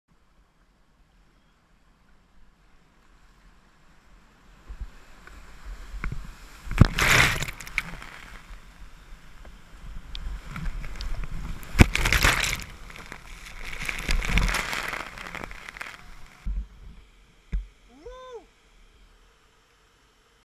mamanya
Маманя. Чувство когда подплываешь к краю среза водопада - несравнимо, это и есть момент истины. твои мысли материализуются в мгновение ока. Нужно лишь успеть поймать то самое, что называют полетом- полётом мысли!)